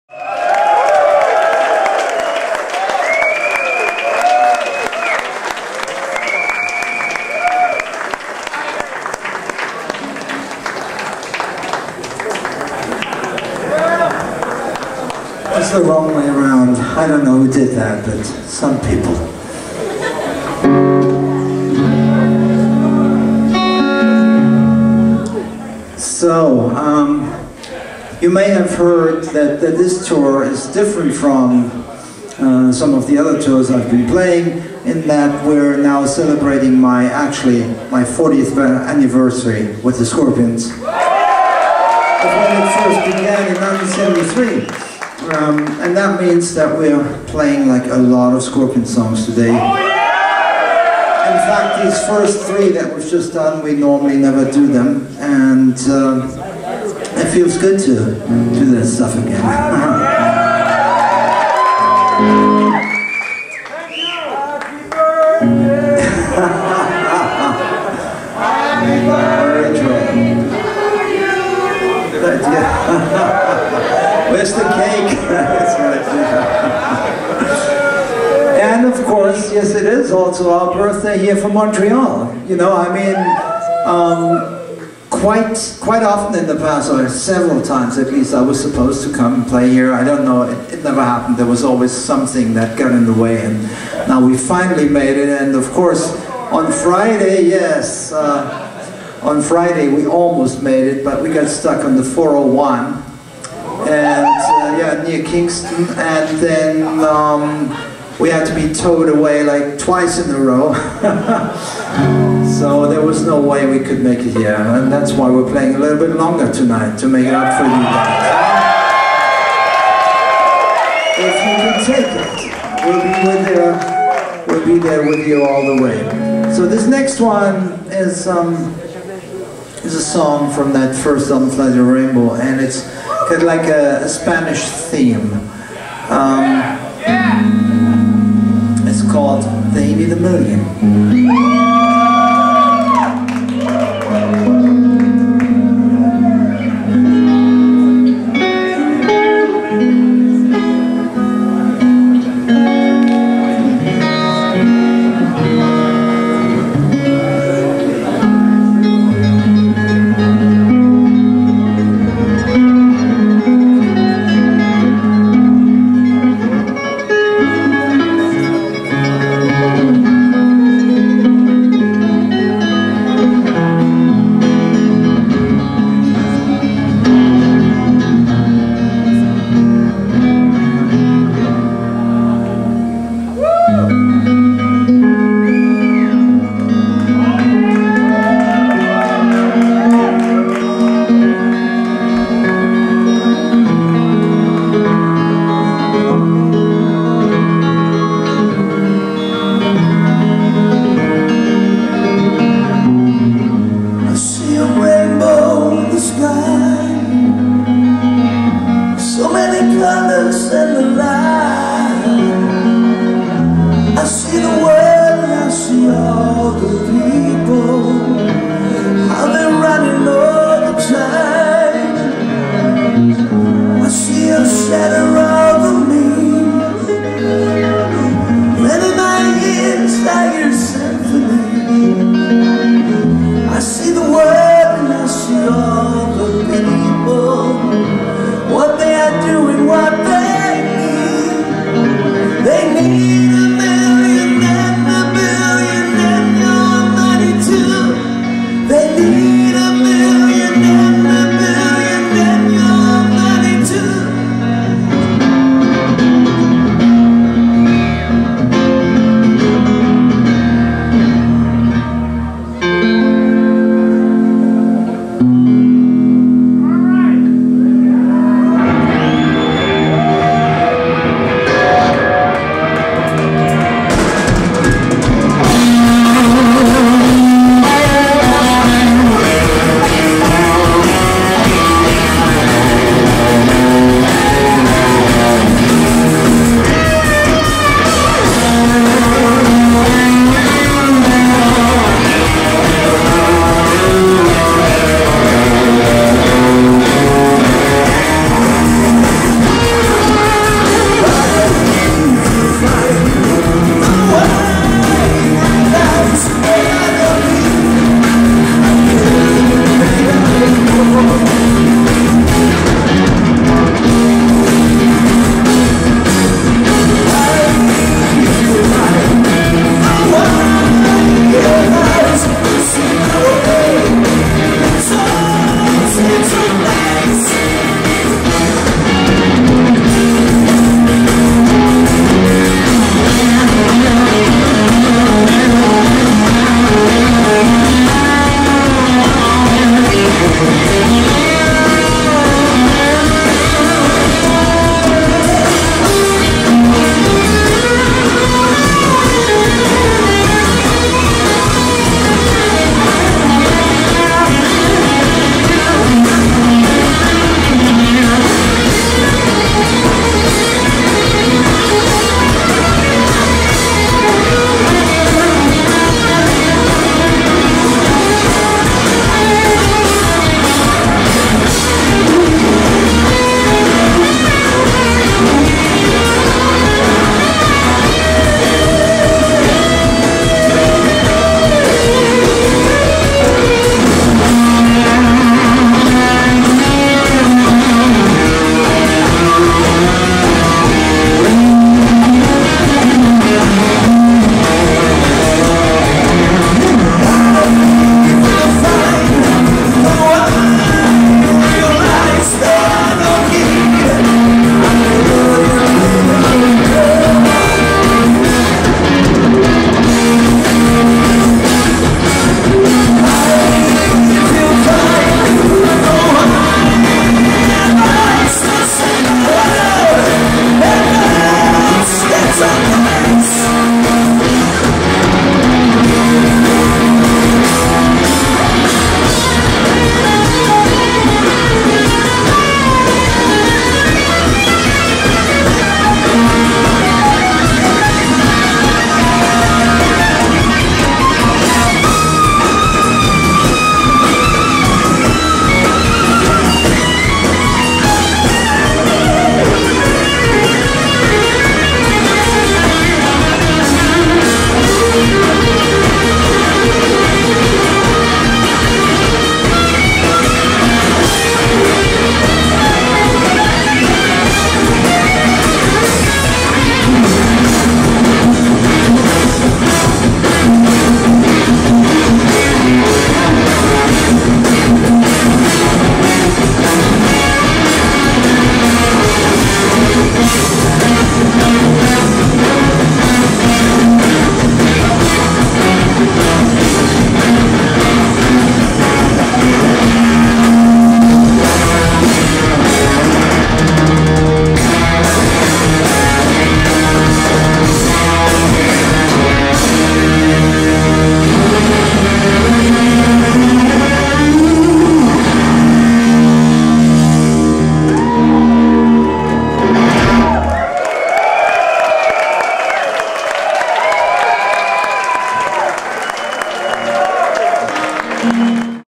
ULI JON ROTH Intro 40th SCORPIONS Anniversary LeNational Montréal 2013
Ex Scorpions German Neo Classical / Metal Guitarist Virtuoso ULI JON ROTH live
Monday February 11 2013 Montreal Quebec Canada...After the Blizzard!...